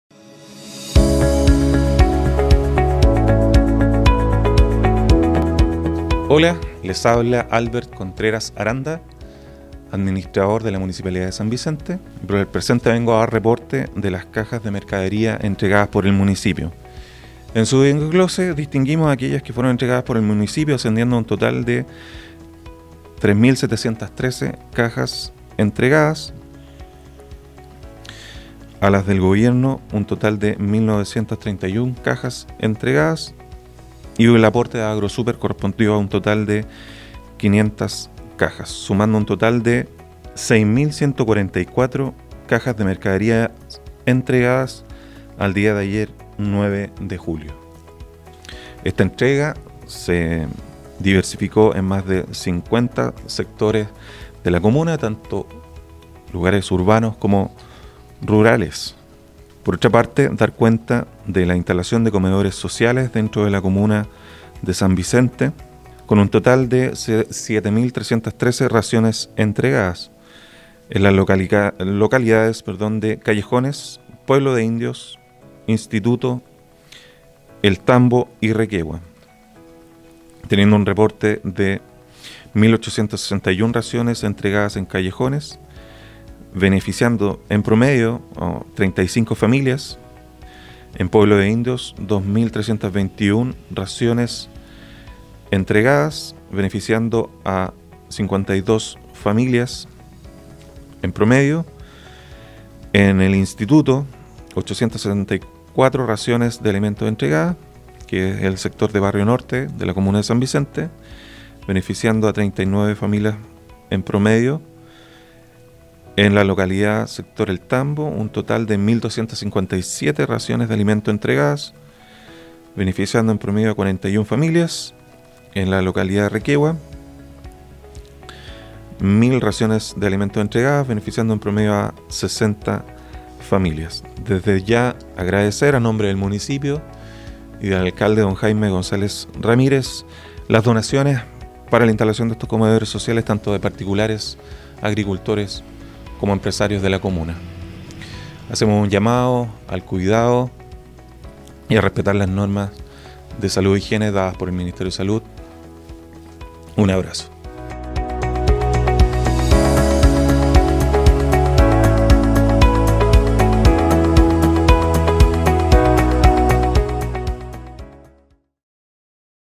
BALANCE SOBRE ENTREGA DE AYUDA SOCIAL ACTUALIZADO AL 9 DE JULIO
El administrador municipal, Albert Contreras, realiza un nuevo balance de la entrega de ayuda social, correspondiente a cajas de alimentos y raciones de almuerzos en comedores solidarios, en informe actualizado al jueves 9 y miércoles 8 de julio, respectivamente.

En dicho reporte, se da cuenta de las cajas de alimentos entregadas con recursos municipales, donación de Agrosuper y fondos del Gobierno Regional llegando a un total de 6.144 cajas a la fecha.

Por su parte, la suma de raciones de almuerzos distribuidos en los cinco comedores solidarios asciende a las 7.313.